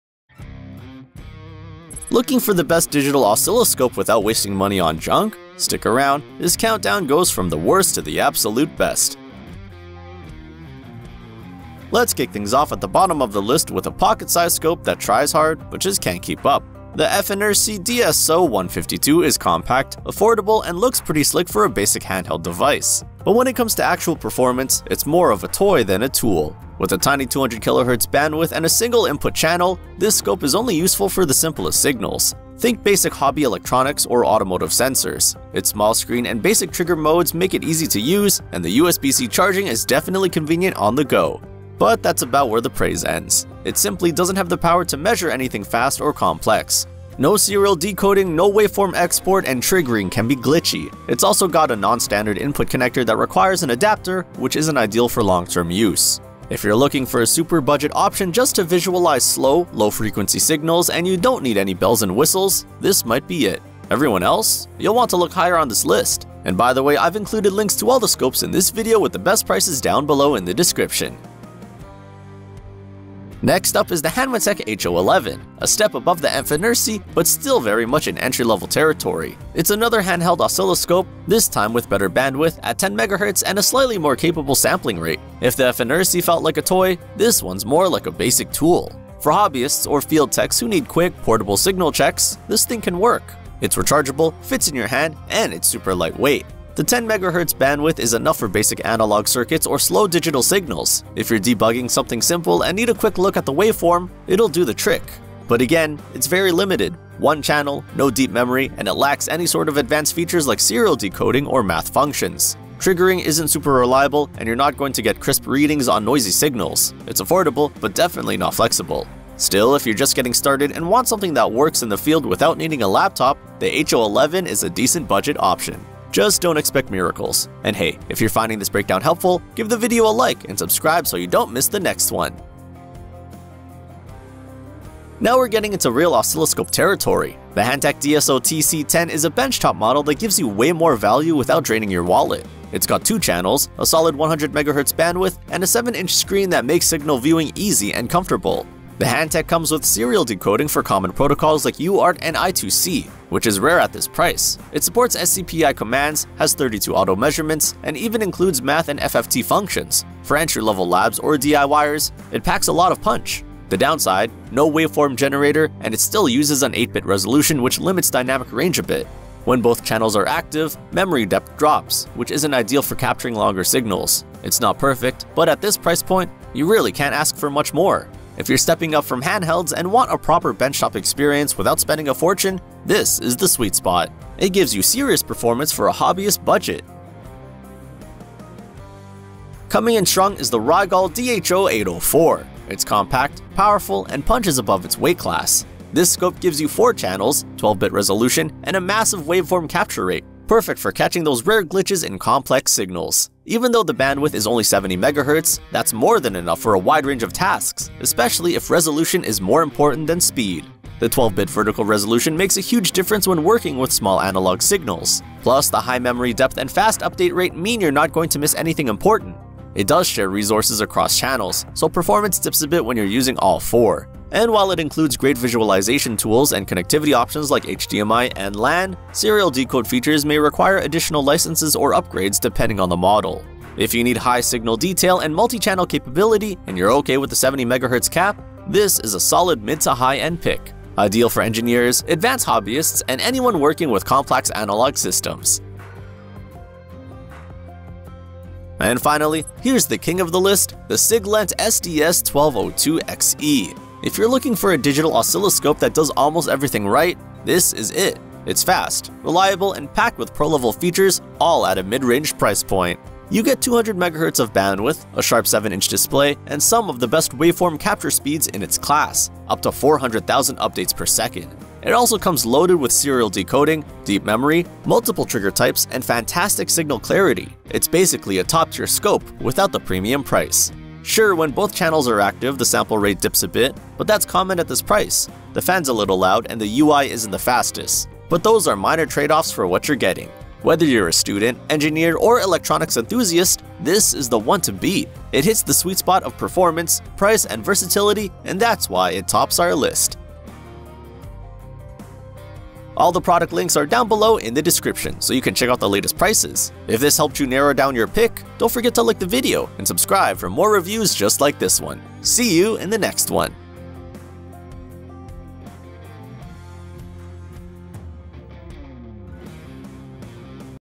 TOP—5. Best Digital Oscilloscopes 2025
FNIRSI DSO152

We tested and ranked the most talked-about digital oscilloscopes—from cheap handhelds to powerful benchtop models. Find out which one gives you the best bang for your buck!